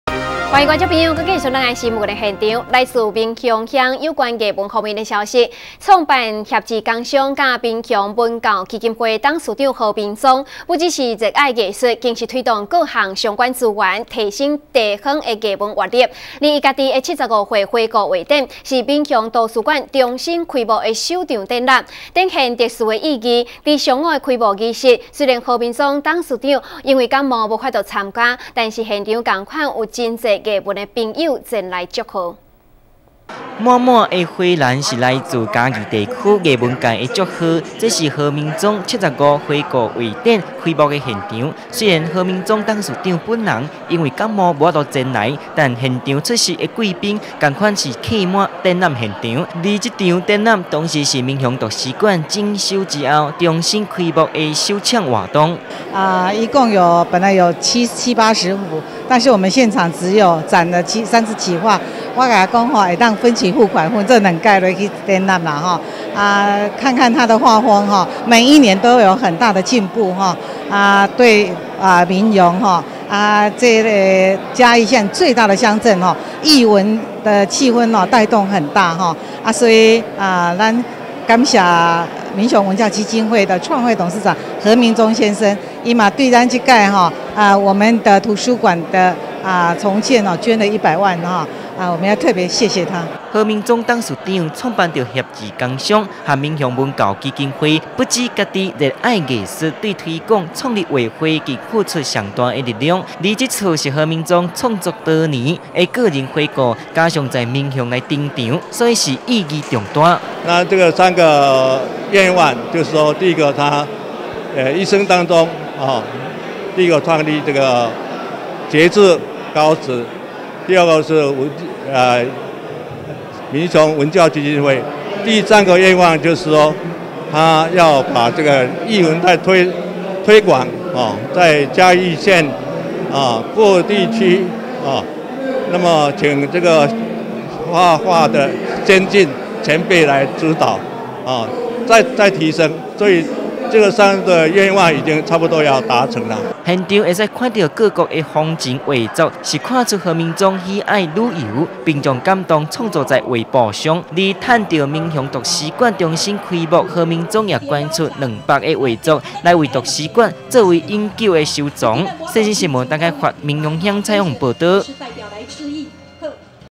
世新新聞 何明宗75回顧畫展 藝文界齊祝賀
何明宗75回顧畫展 藝文界齊祝賀

創辦協志工商和民雄文教基金會的董事長何明宗，不只熱愛藝術，更推動各項相關資源，提昇地方藝文活力，而他自己的75歲回顧畫展，是民雄圖書館重新開幕的首場展覽，是展現特殊意義，在上午的開幕儀式，雖然何明宗董事長因為感冒，無法參加，但現場一樣有很多藝文朋友前來參加。